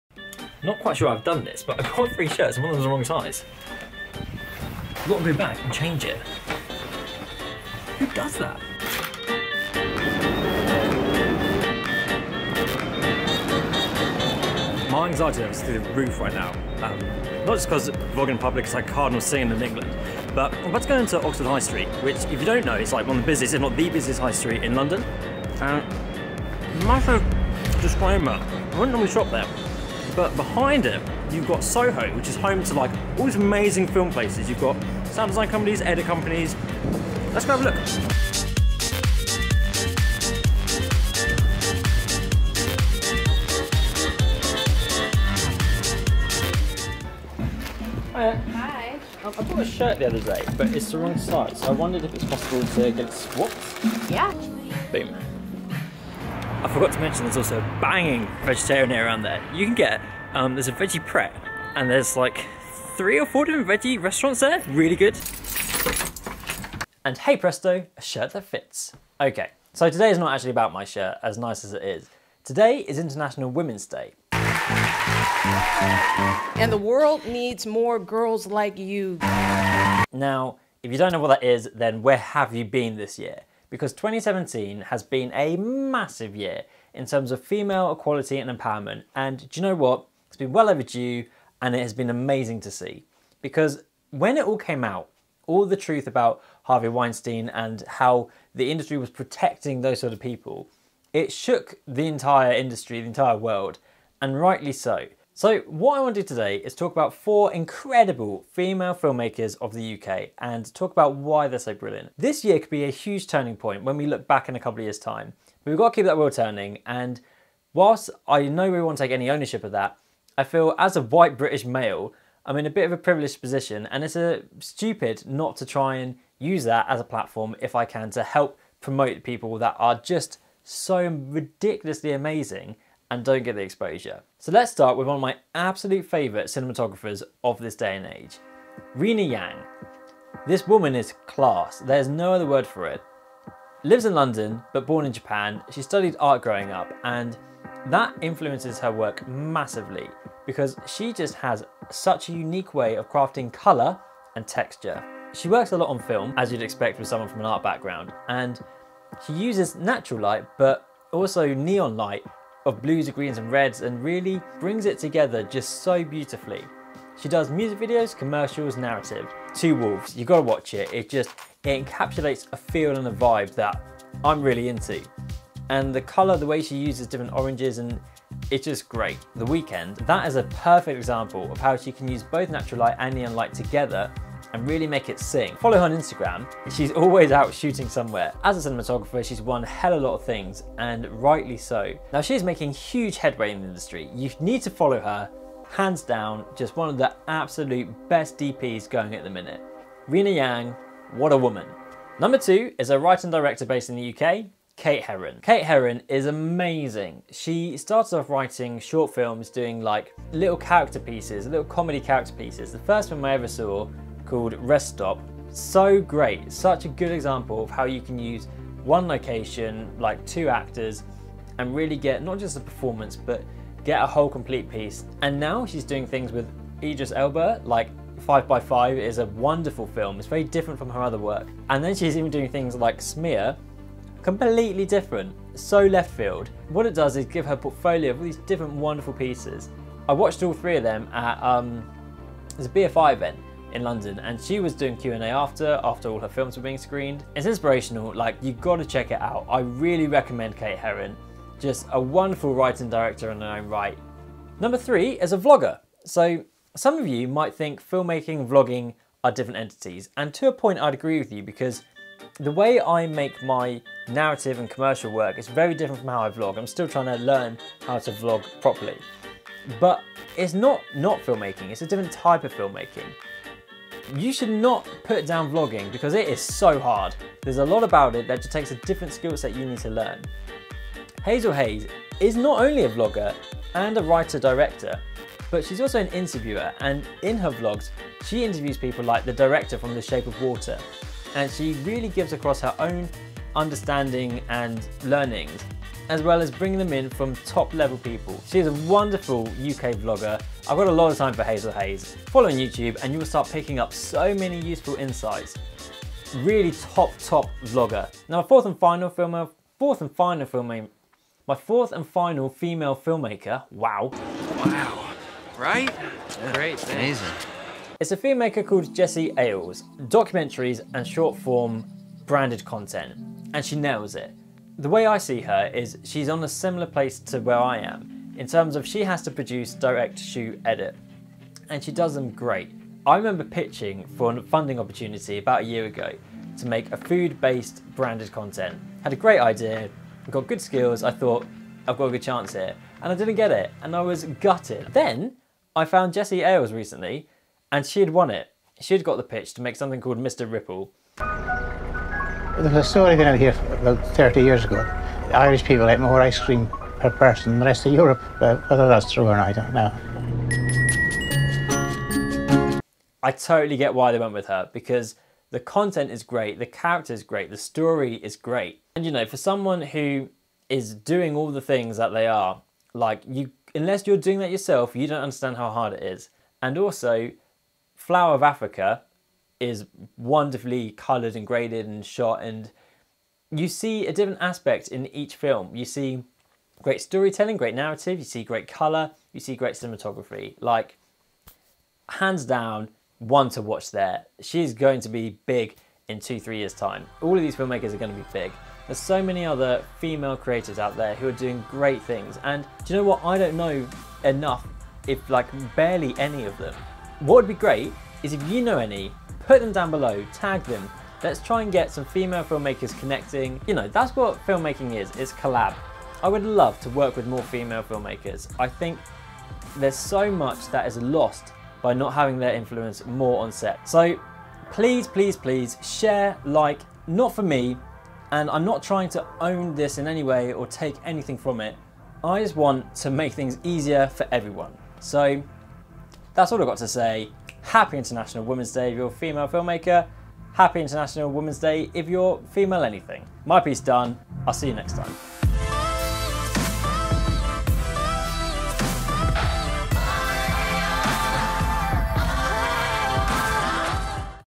The future female filmmakers of 2018 (UK)
Who are the future female filmmakers of the UK film industry?

Women in the film industry are hugely underrepresented, and hugely talented, so it's about time we shined a spotlight on some of the up and coming female filmmakers of the UK, and its only right as it is international women's day today!

These are just some of the women out there we admire, comment below and tell us who you think is a brilliant female filmmaker, or tell us about yourself if you ARE that brilliant female filmmaker.

Rina Yang: An incredible cinematographer, known for her outstanding work in the music video, narrative and commercial work. London based, she's worked for clients such as Nike and Adidas, musicians like Bjork, and won numerous cinematography awards.

Kate Herron: Writer and director of short comedies and dramas, Kate Herron brings female talent to the fore, both in her own ability, and the characters she writes as well as the casting that brings them to life. Releasing her new film today, we know it's going to be a good one! Films include Smear, 5x5 and Rest Stop.

Hazel Hayes: A persona you can't help but love, a talent you can't help but admire, and interviewees you can't help but envy. As one of the UKs most successful YouTubers, Hazel Hayes not only writes and directs her own brilliant content, but she interviews some of the industry's most prolific figures, including multiple Oscar winners! And we'd be amiss if we didn't mention the fact she has a stunning singing voice. Is there anything she can't do?

Jessie Ayles: An extraordinary filmmaker on the rise, shooting branded content and short form documentary, winning MoFilm pitches and definitely making massive headway into the commercial scene. An inspirational figure for any independent filmmakers out there who rely on themselves.